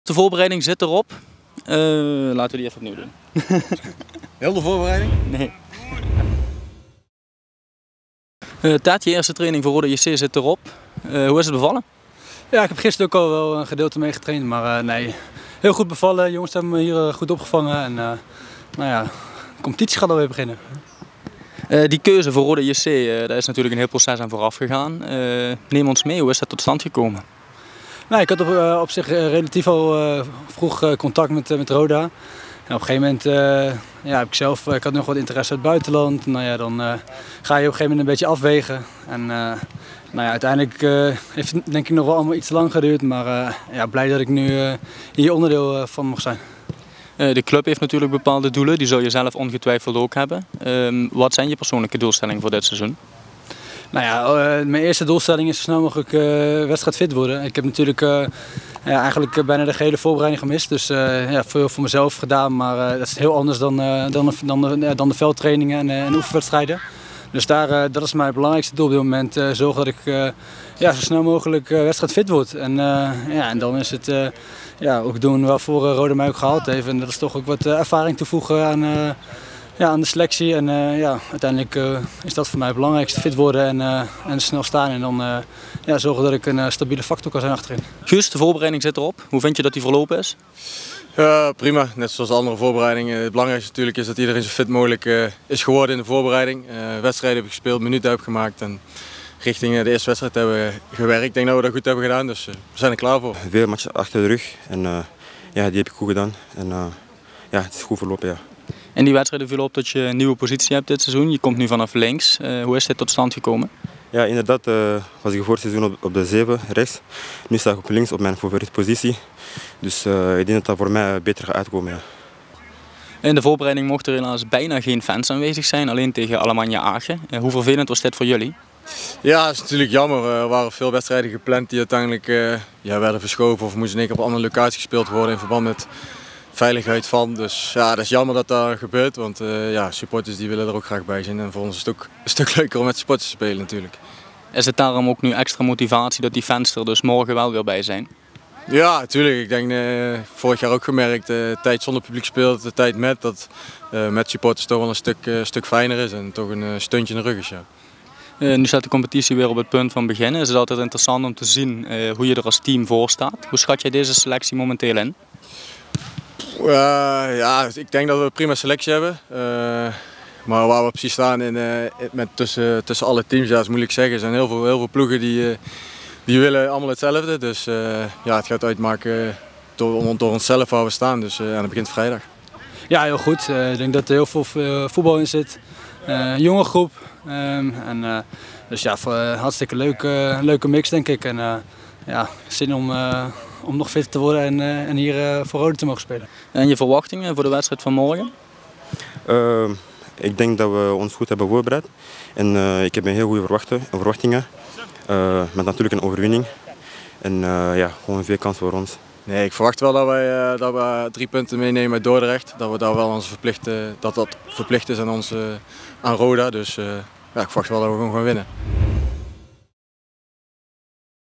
VOORBESCHOUWING | FC Dordrecht - Roda JC Kerkrade | 5 augustus 2022
In de eerste voorbeschouwing van dit seizoen blikken we met kersverse aanwinst Ted van de Pavert en met Guus Joppen en Bryan Limbombe voor op het duel van morgenavond tegen FC Dordrecht. Met Van de Pavert blikken we ook kort terug naar zijn keuze om een transfer naar Roda JC Kerkrade te maken.